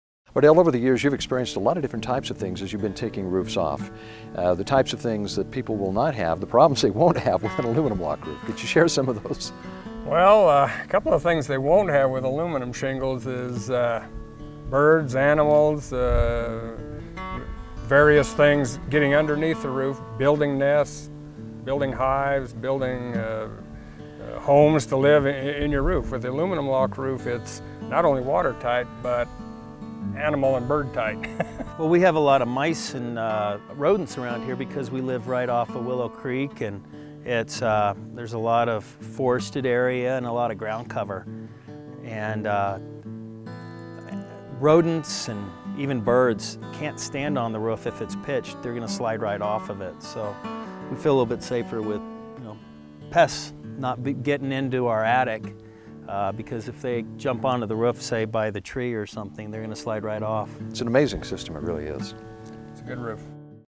Pest Proof! Humane! Aluminum Lock Roofing
Squirrels! Birdsnests!  Safely Begone! Customers tell their story.  Watch for the cute squirrels in the trees...  ( :

See all our videos on our channel!